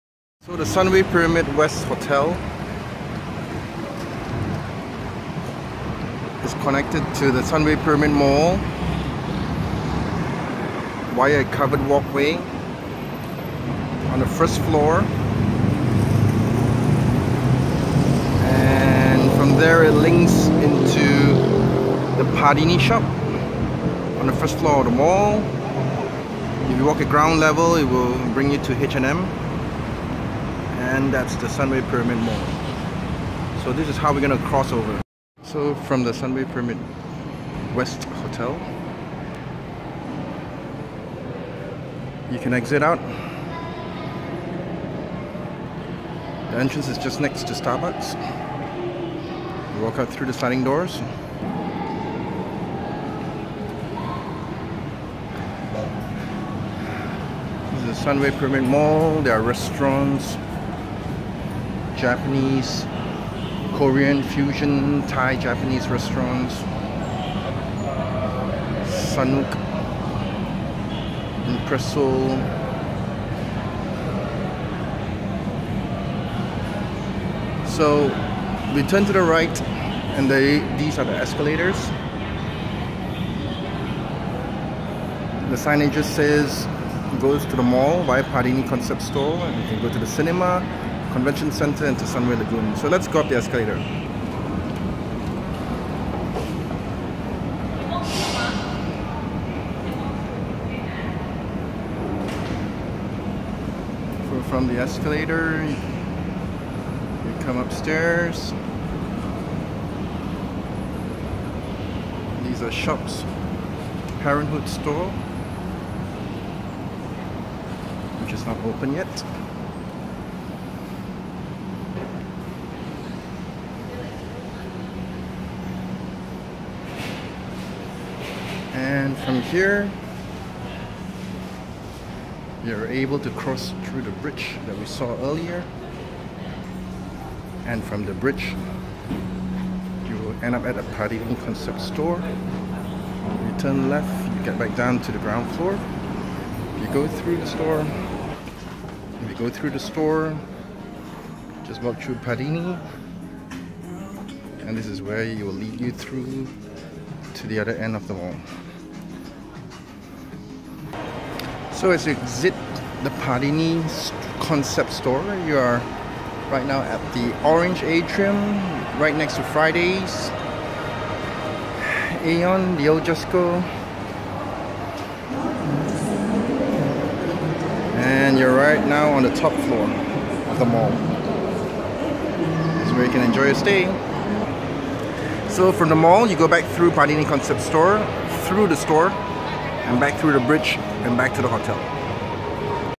Sunway Clio Hotel connects to Pyramid Mall via Covered Bridge (formerly Sunway Pyramid Hotel West)
This is a short video showing how Sunway Clio Hotel (formerly Sunway Pyramid Hotel West) connects to Sunway Pyramid Mall via the Covered Bridge walkway on the first floor of the hotel & mall.

We will video a walk thru from the Hotel Lobby to the Sunway Pyramid Mall, exiting at the Padini Concept Store.